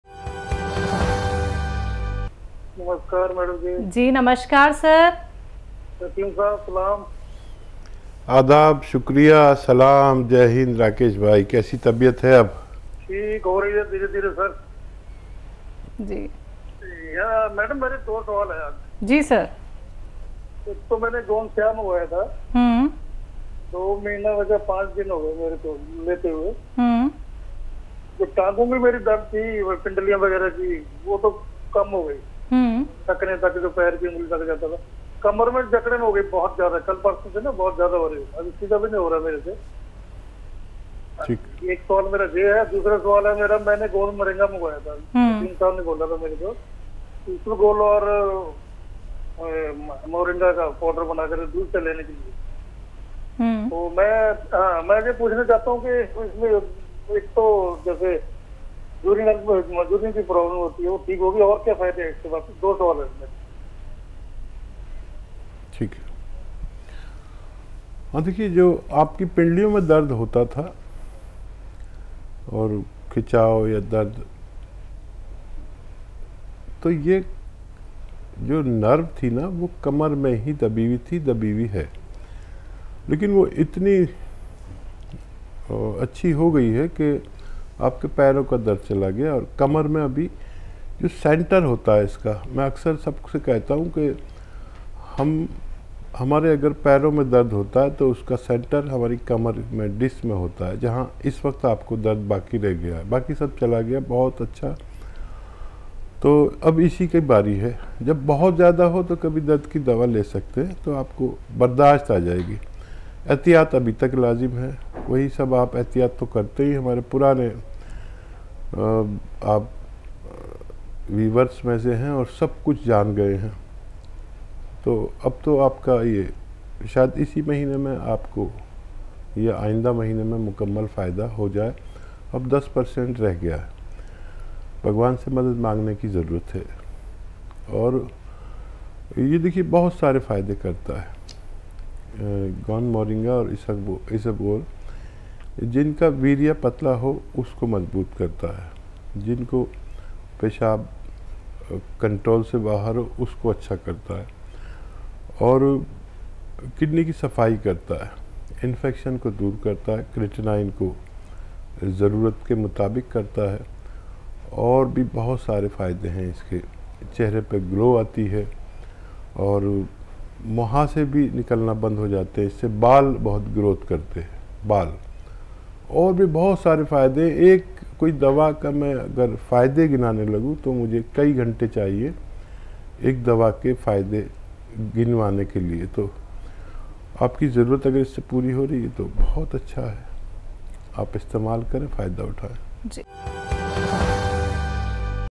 गोंद मोरिंगा के इतने फायदे जानकर आप भी हैरान हो जायेंगे | Hakim Suleman Khan | Sadhna TV
Thanks.